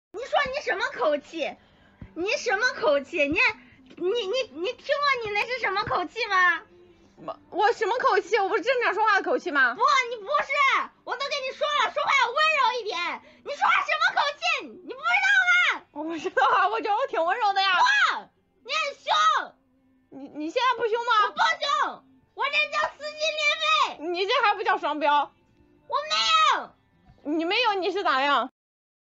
[EngSub] "Double Standards" - Hu XiaoHui, Liu ShuXian 胡晓慧 刘姝贤 【奶包】SNH48/BEJ48
Hu XiaoHui (red) and Liu ShuXian (green) have a little argument on a live stream

The audio live stream is from 20200314